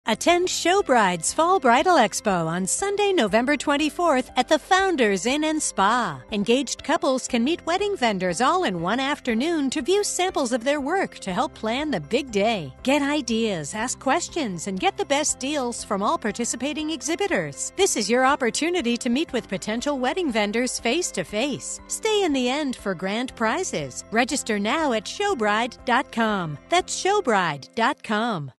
Fall Bridal Expo 11/24 at the Founders Inn & Spa by Showbride
Attend Showbride's Fall Bridal Expo on Sunday, November 24th at The Founders Inn & Spa. Engaged couples can meet wedding vendors all in one afternoon to view samples of their work to help plan the big day! Get ideas, ask questions & get the best deals from all participating exhibitors. This is the opportunity to meet with potential wedding vendors face-to-face. Stay in the end for grand prizes. Register now at showbride.com!